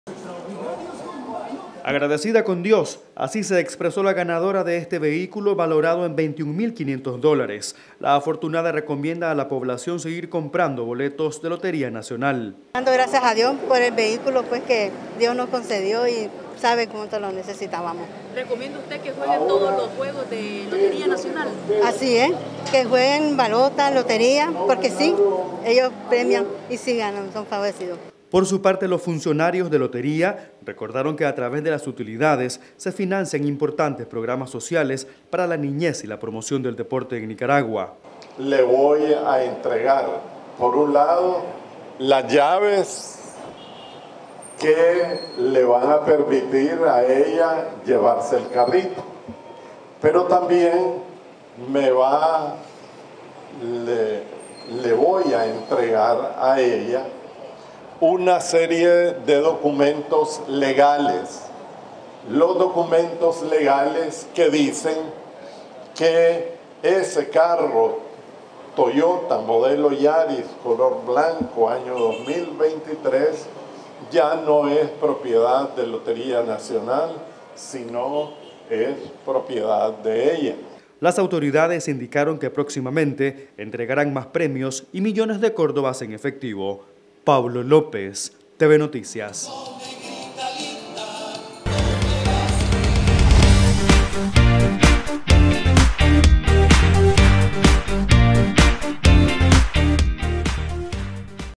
Capitalina gana auto del año en el "Juego Balotas" de Lotería Nacional
Doña Rosa Estela Reyes Lara es la feliz ganadora de un automóvil en el juego Balotas de Lotería Nacional. El vehículo entregado está valorado en 21 mil 500 dólares y es parte de los premios de este sorteo.